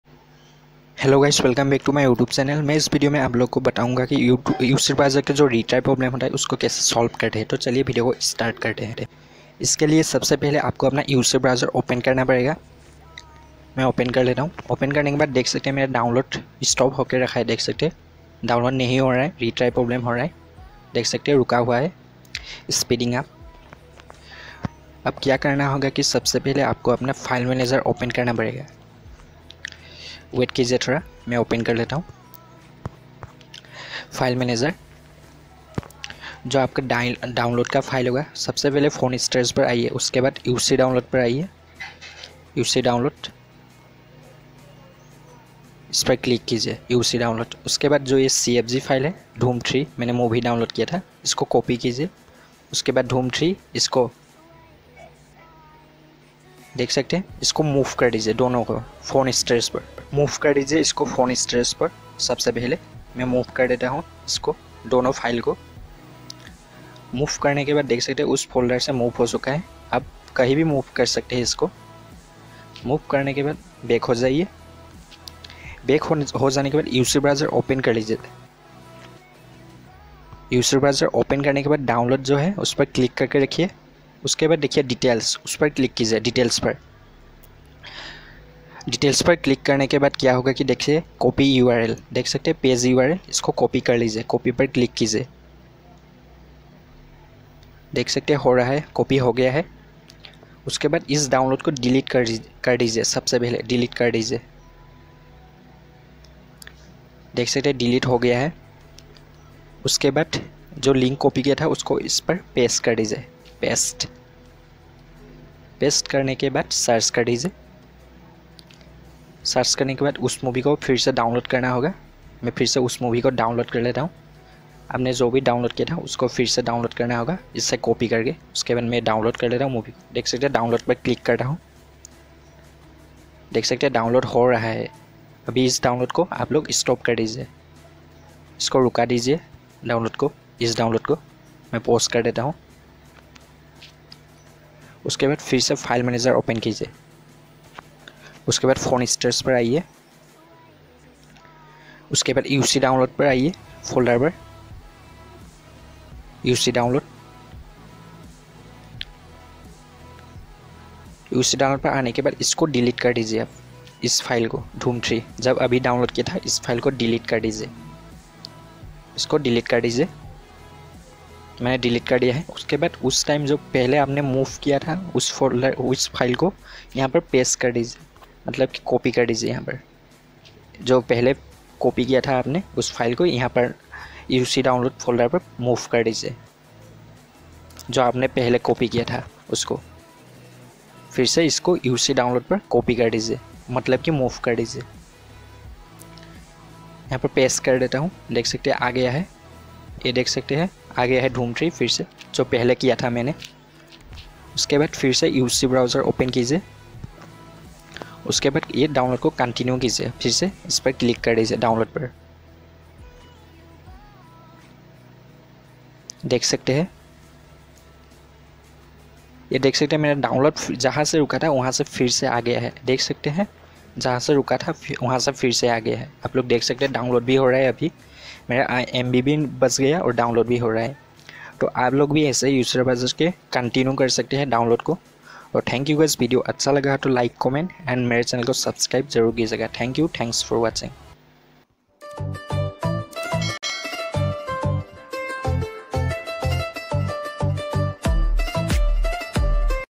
How To Solve Uc Browser Download Retrying Problem 2023 in just 2 minute ll 100% Working Trick ll
How To Solve Uc Browser Retrying Problem,